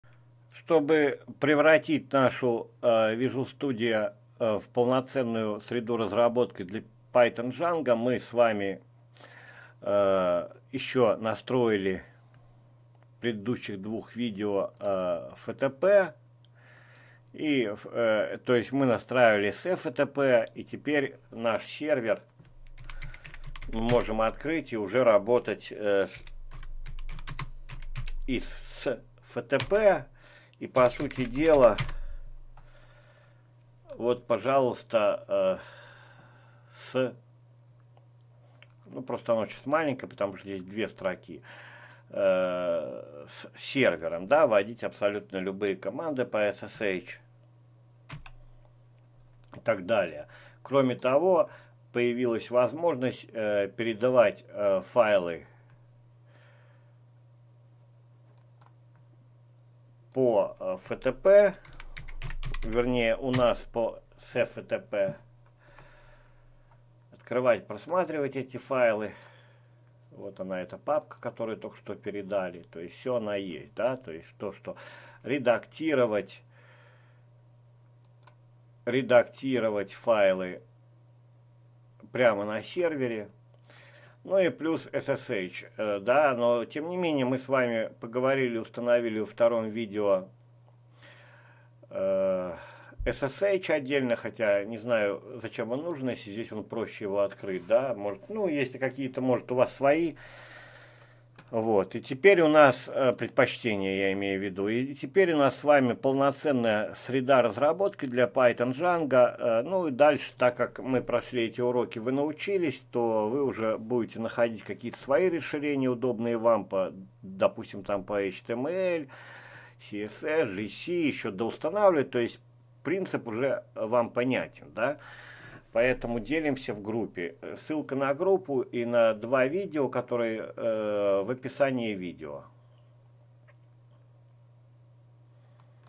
VS studio code настройка SFTP/SSH.
Ещё два урока, настройка
SFTP + SSH
и настрока SSH

На этом мы заканчиваем серию уроков, я думаю вы приобрели первый опыт и в процессе работы, сможете уже самостоятельно выбрать для себя расширения для
более удобной работы html, js и другие.

Прошу вас делится с ребятами в этом разделе, что вам нравится, что вы установили и так вместе мы быстрее сделаем из Vs studio очень крутой инструмент, для работы с разными языками.